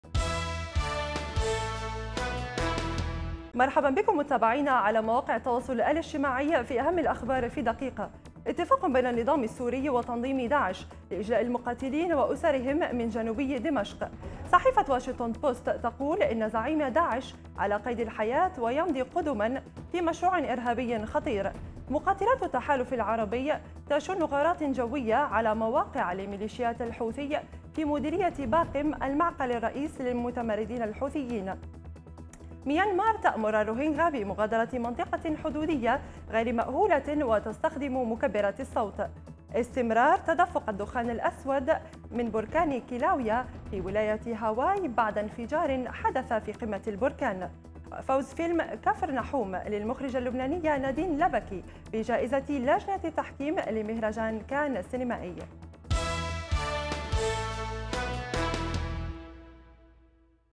اتفاق بين النظام السوري وتنظيم داعش لإجلاء المقاتلين وأسرهم
- اتفاق بين النظام السوري وتنظيم داعش لإجلاء المقاتلين وأسرهم من جنوبي

- صحيفة واشنطن بوست تقول إن زعيم داعش على قيد الحياة ويمضي قدما في مشروع إرهابي خطير

- مقاتلاتُ التحالف العربي تشن غاراتٍ جوية على مواقعَ لميليشياتِ الحوثي في مديرية باقم، المعقلِ الرئيس للمتمردين الحوثيين

- ميانمار تأمر الروهينغا بمغادرة منطقةٍ حدودية غير ِ مأهولة وتستخدم مكبرات الصوت

- استمرار تدفق الدخان الأسود من بركان "كيلاويا" فى ولاية هاواي بعد انفجارٍ حدثَ فى قمة البركان

- فوز فيلم "كفرناحوم" للمخرجة اللبنانية نادين لبكي، بجائرةِ لجنة التحكيم لمهرجان "كان" السينمائي

برقية_أخبار_الآن تقديم: ناهد العثماني